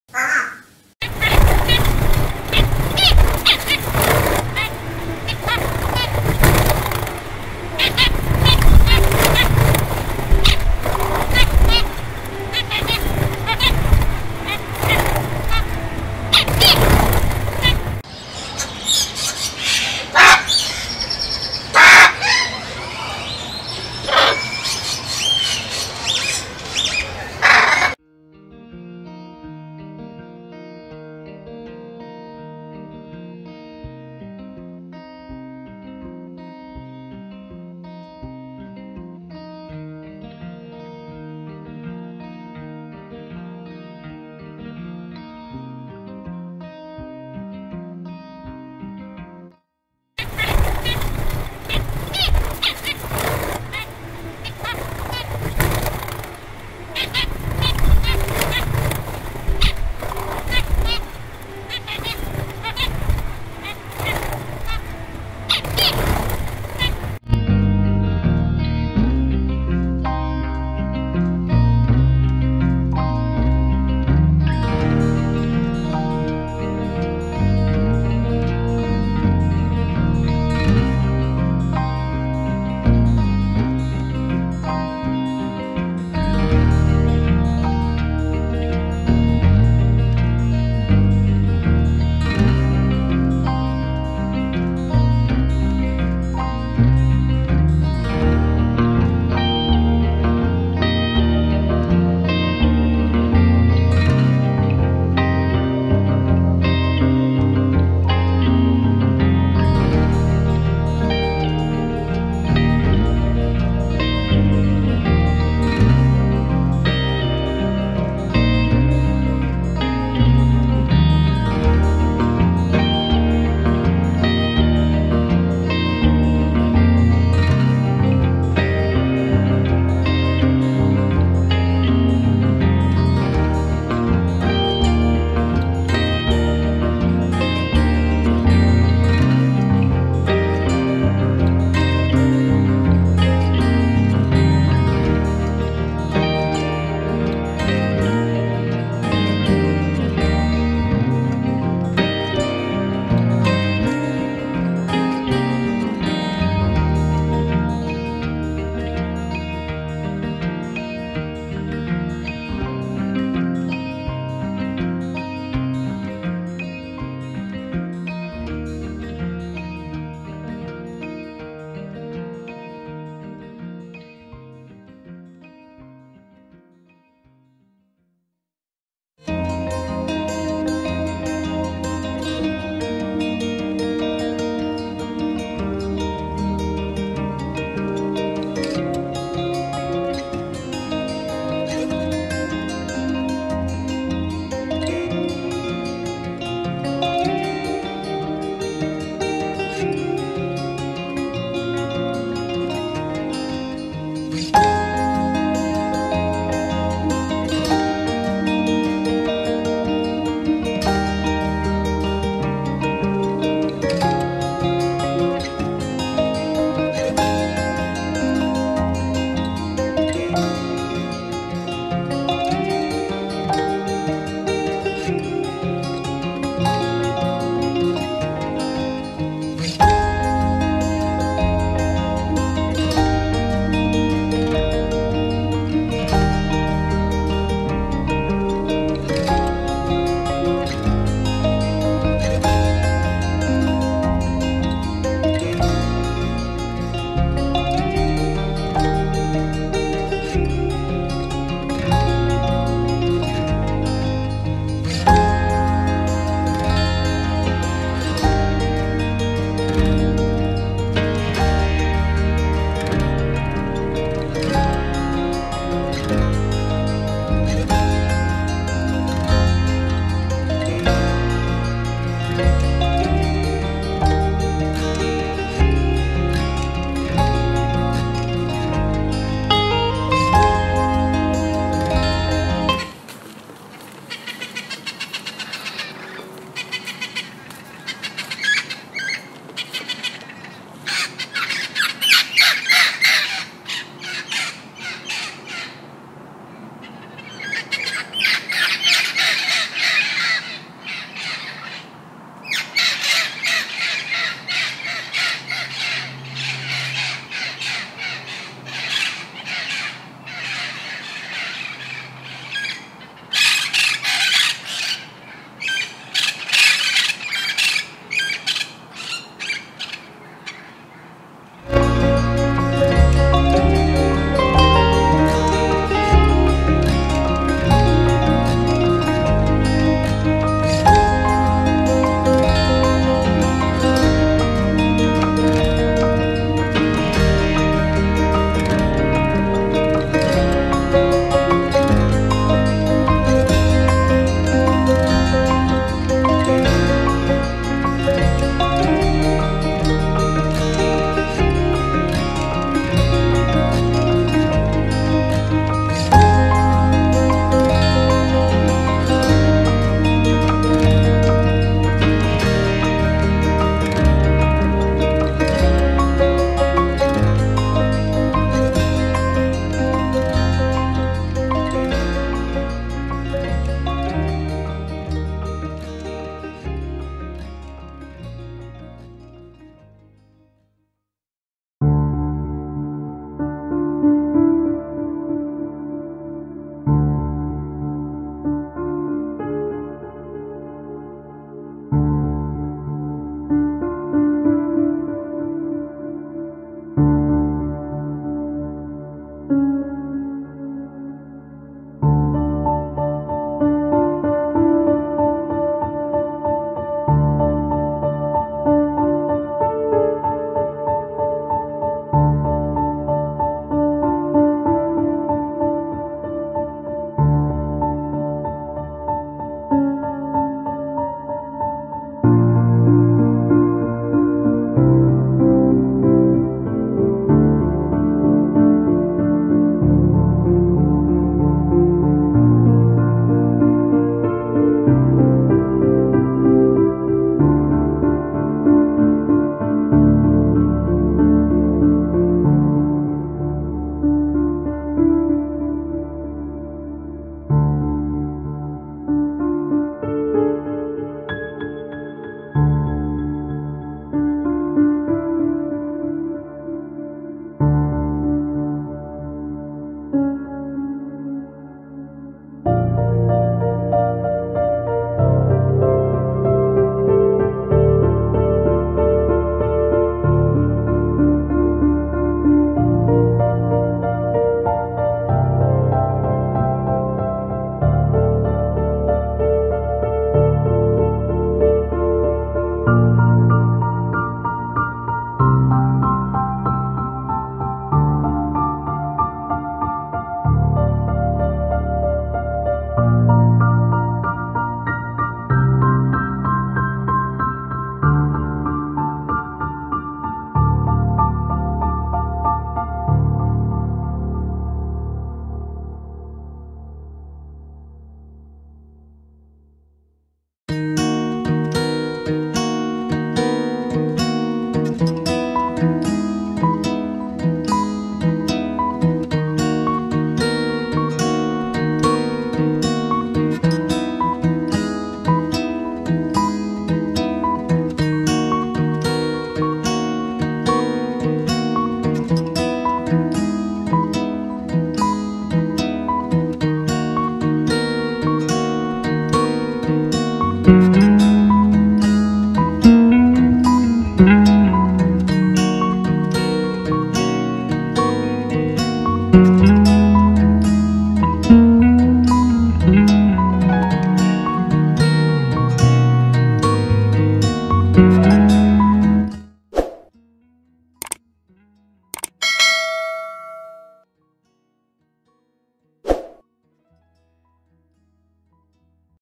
Funny Parrots Video Compilation - Cute Moments of Animals and Birds
Best funny parrots video compilation cute moments 2021. Funny and cute 🦜 videos that you will enjoy.